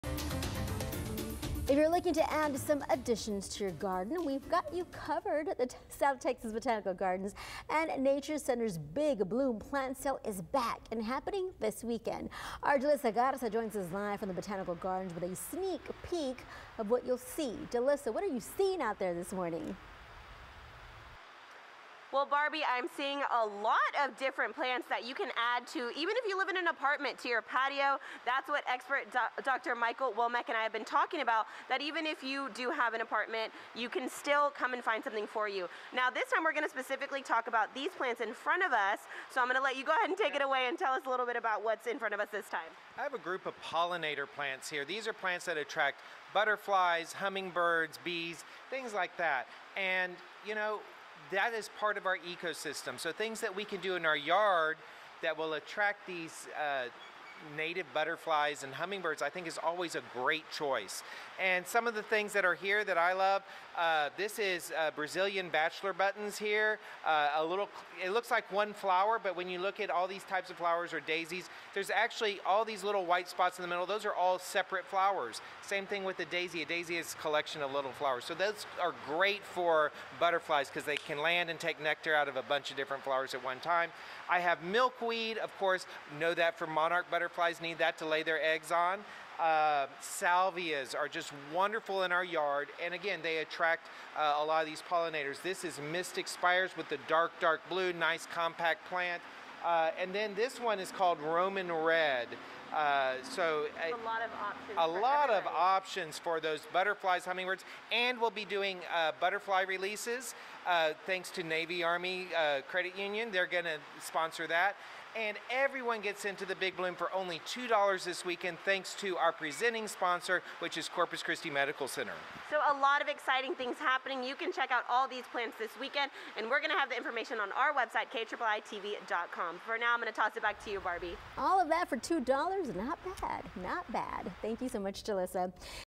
Big Bloom Plant Sale returns to the South Texas Botanical Gardens
Members of the Botanical Gardens can get into the event for free and take advantage of a members-only shopping period between 9 a.m. and 10 a.m.